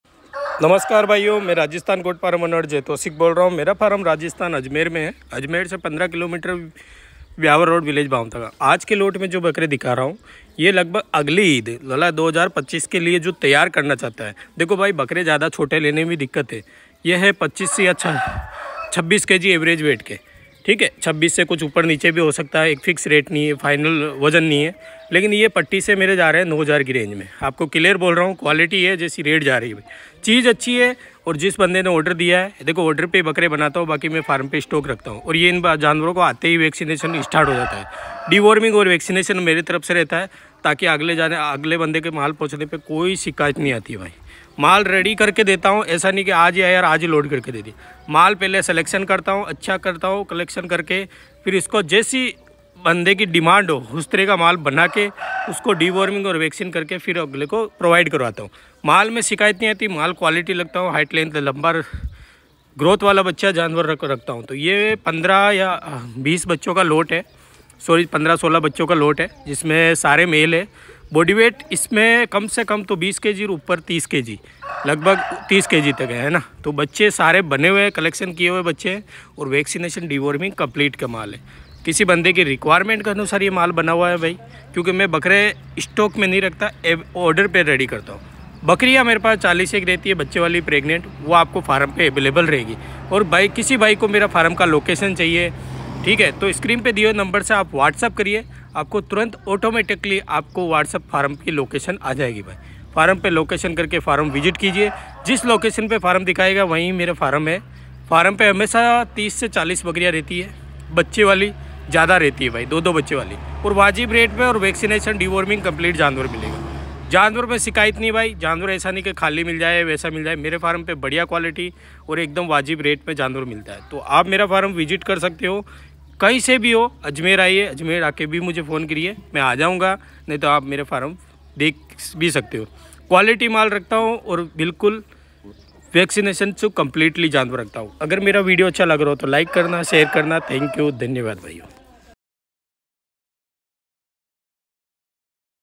सिरोही बकरे 27 kg ओसत वजन 9000 की पट्टी से (राजस्थान गोट फ़ार्म अजमेर ) 9783341533
Rajasthan goat farm Ajmer (jay toshik) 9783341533

Google map